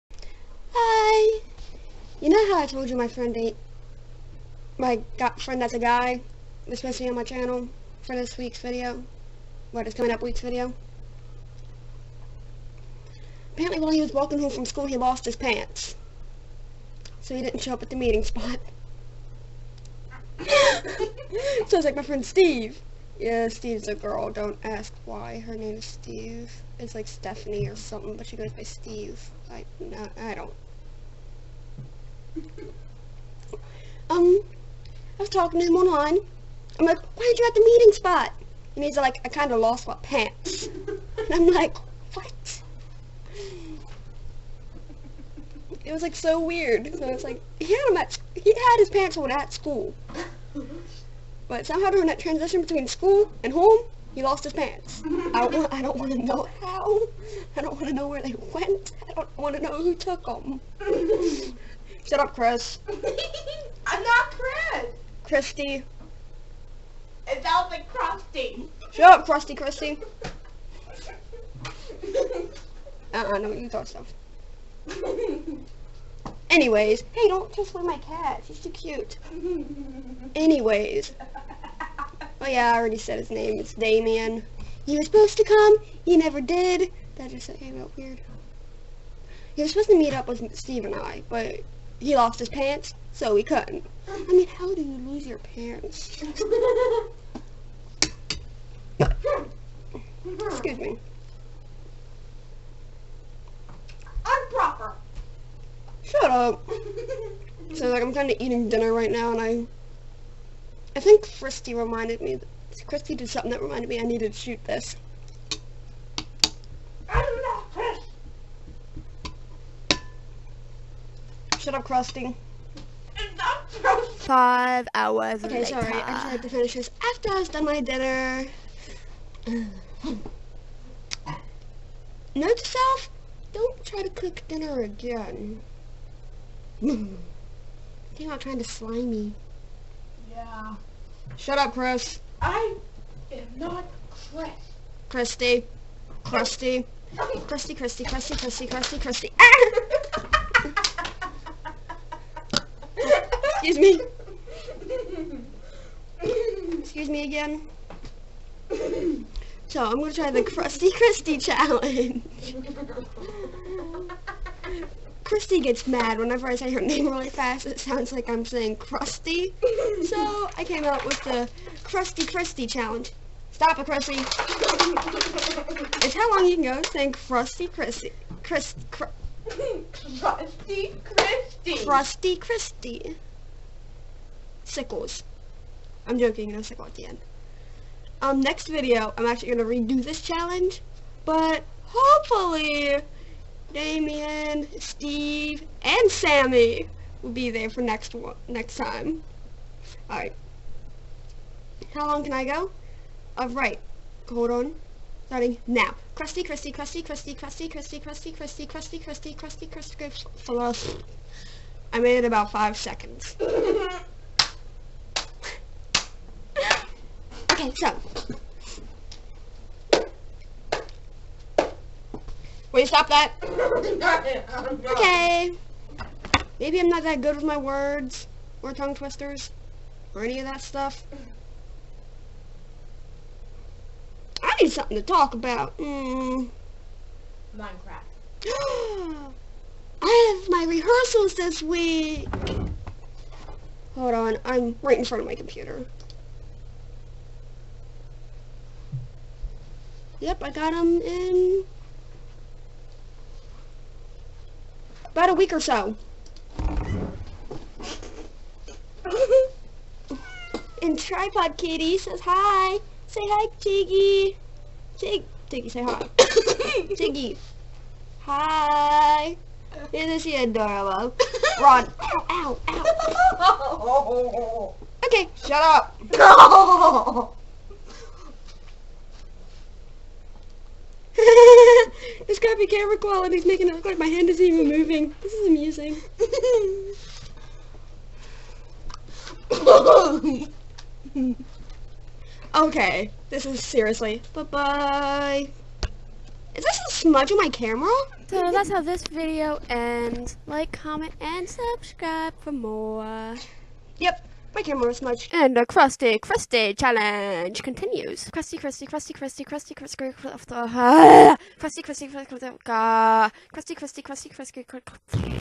Crusty Christy
ATTENTION! IF YOU ARE WATCHING THIS.. I WANT YOU TO TRY THE CRUSTY CHRISTY CHALLANGE. (Don't worry, Christy doesn't mind.) AND MAKE IT A VIDEO RESPONSE TO THIS VIDEO~ Christy and I want to see who can beat my fabulous five seconds!! So, I told Christy when she got older she should make a channel and call it The Crusty Christy. Yeah, she didn't like the idea.. Anyways. I finally got my microphone back. So I decided to record narrations.. I like it. Once Jay and I can finally get some webcams she will be back on the channel and I'll have a better camera. But that won't be until June.

For more Crusty Christy. And Me, the fabulous Alec! Next video Damian should have his pants and you will be able to meet more of my fabulous friends!